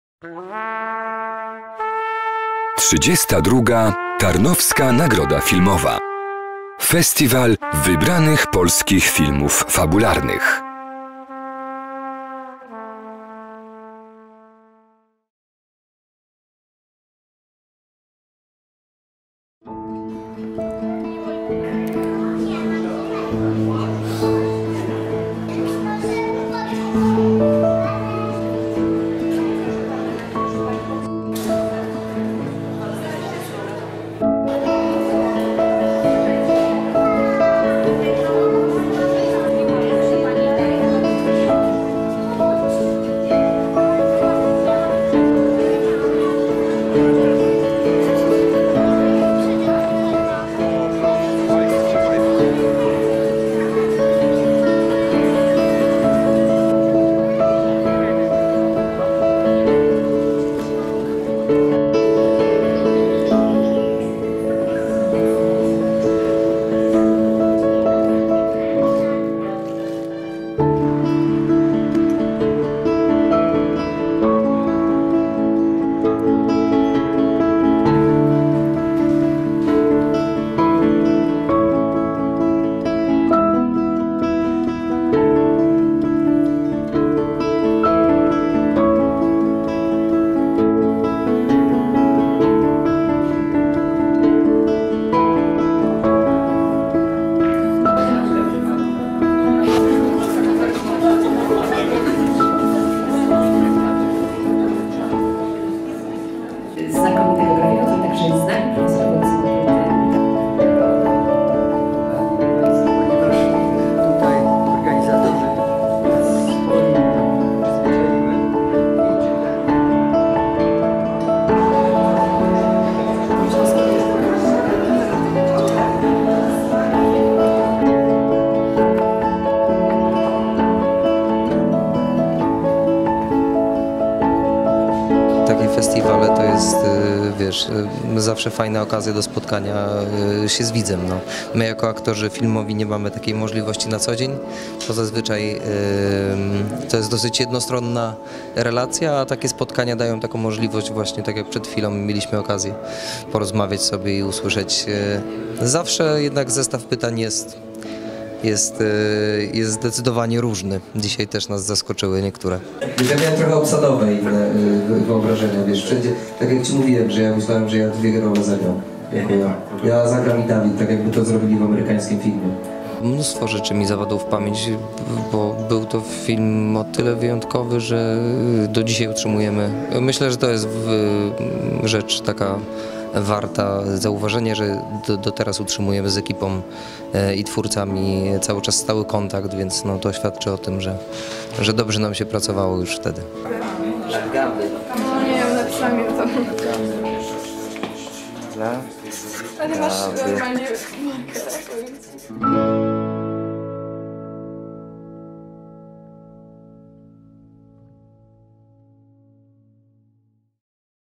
32. Tarnowska Nagroda Filmowa - Dzień 2, 22.04.18
Poranek filmowy dla dzieci, zwiedzanie wystawy "Mieczysław Wasilewski. Plakaty" (BWA), spotkanie z twórcami filmu "Cicha noc" - Kino Marzenie, 22.04.18

Realizacja: Telewizja ZVAMI dla Tarnowskiego Centrum Kultury